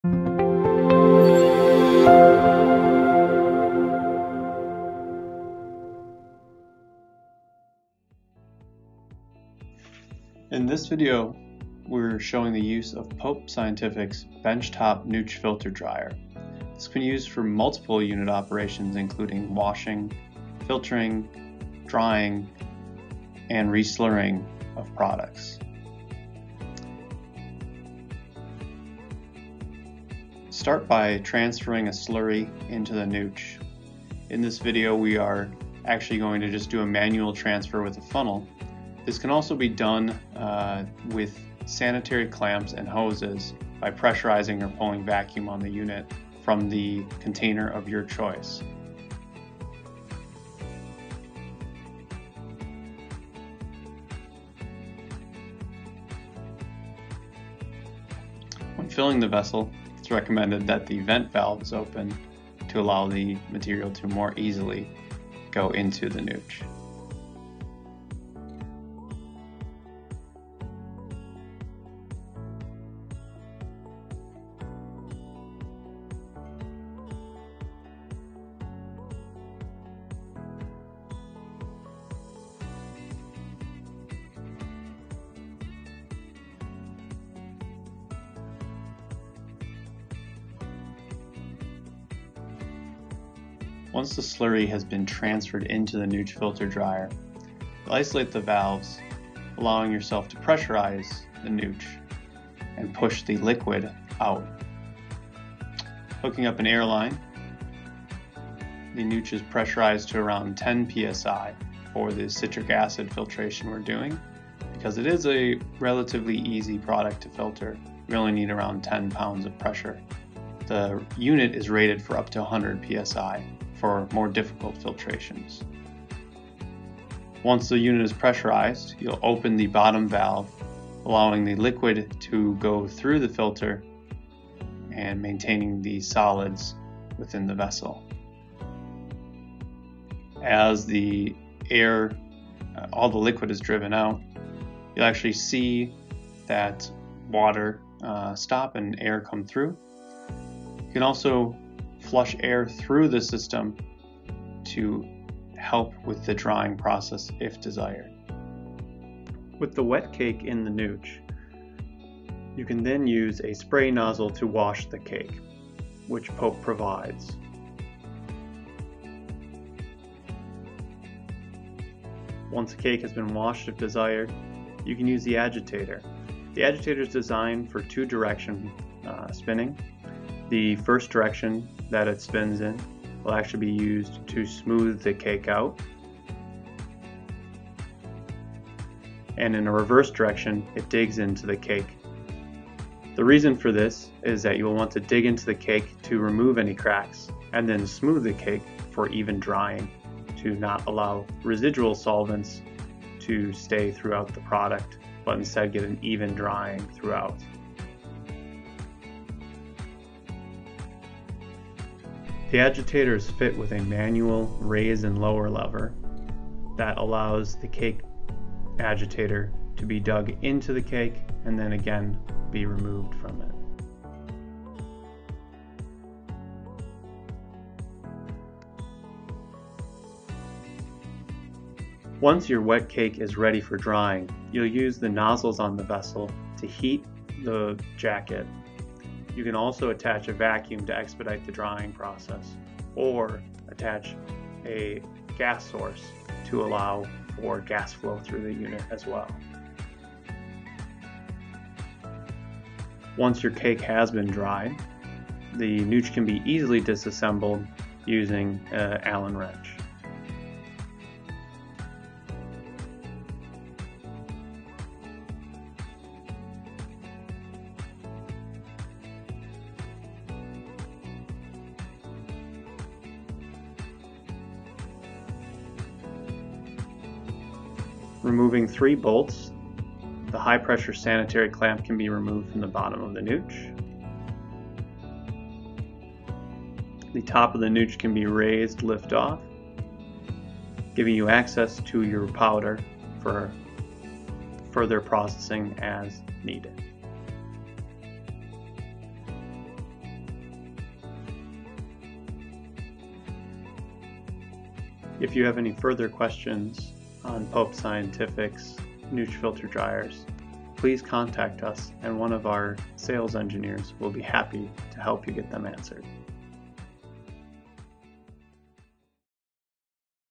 Operating a Benchtop Nutsche Filter Dryer
In this video, @PopeScientificInc Sales and Process Engineer, Ben Kultgen demonstrates how a Benchtop Nutsche Filter Dryer (also known as ANFD or NFD) is operated. Pope’s benchtop Nutsche can perform multiple operations such as washing, filtering, drying, and reslurrying of products.  The operation begins with transferring a slurry to the Nutsche Filter.  The valves are then isolated, pressurizing the Nutsche, and pushing the liquid out. This unit is rated up to 100 PSI.  The bottom valve is opened, allowing for the liquid to flow out of the unit. A spray nozzle is used to wash the cake. An agitator is used to smooth and dig into the cake, which allows for even drying of and removal of residual solvents.  The agitator can be manually raised and lowered for this.  Once the cake is ready for drying, nozzles on the vessel are used for heating the jacket. A vacuum can also be attached be used to expedite the drying or a gas source may be attached to allow for gas flow through the unit. Once the cake is dried, the Nutsche is easily disassembled using an allen wrench. After removing the three bolts, the high-pressure sanitary clamp can be removed from the bottom of the Nutsche, and the top can be lifted off, providing access to the cake material for further processing as needed.